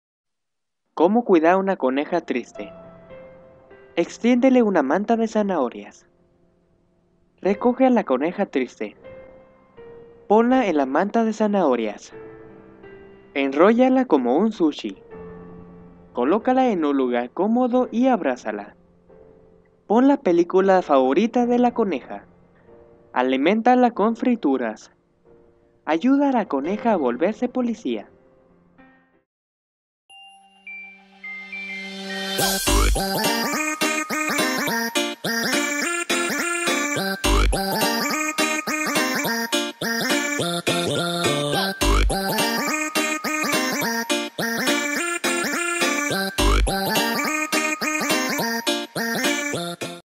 "Como cuidar a una Coneja Triste" | Judy x nick" | Comic Dub |【ESPAÑOL LATINO】
(¯`•._.•█▆▅▃▂ ..Abreme Porfavor ..  ▂▃▅▆█ •._.•´¯)

Hola soy belfandubs y en el dia de hoy les traigo un comic que ami me gusto mucho y espero que les guste

♬ EDICION-ADAPTACION  ✔

EDICION : BelFandubs

ADAPTACION : BelFandubs

VOCES  ≧◡≦

 "Nick: BelFandubs"